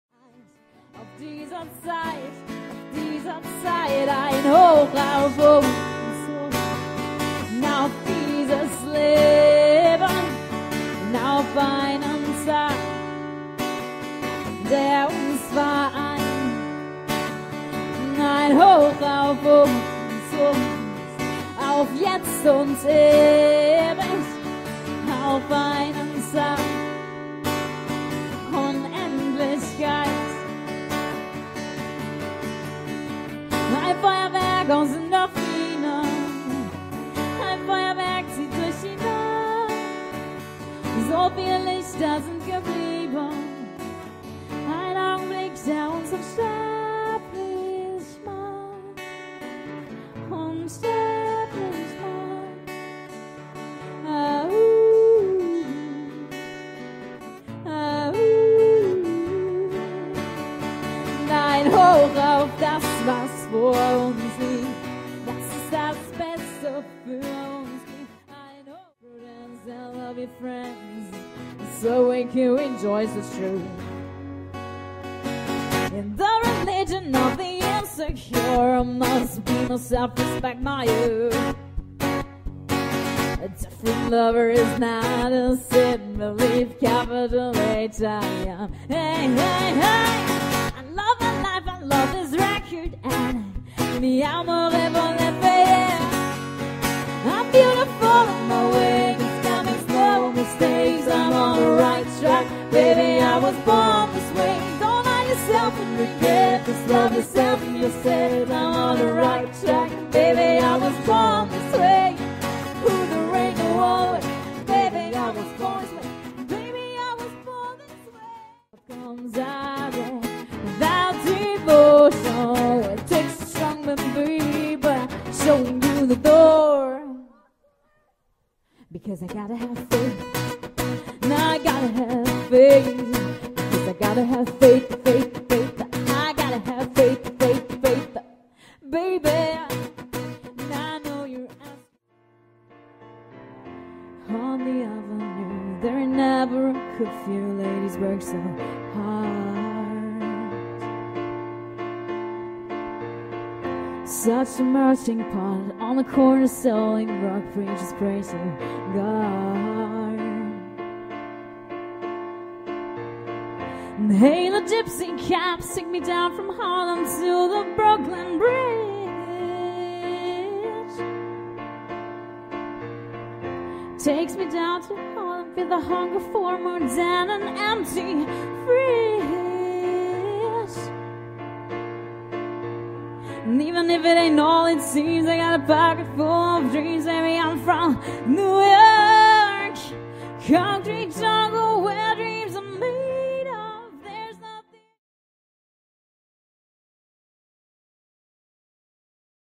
Juno Acoustic - Sportwoche Berg 2014
Hier ein kleiner Zusammenschnitt von unserem Gig an der Sportwoche in Berg mit unserem Juno Acoustic Duo.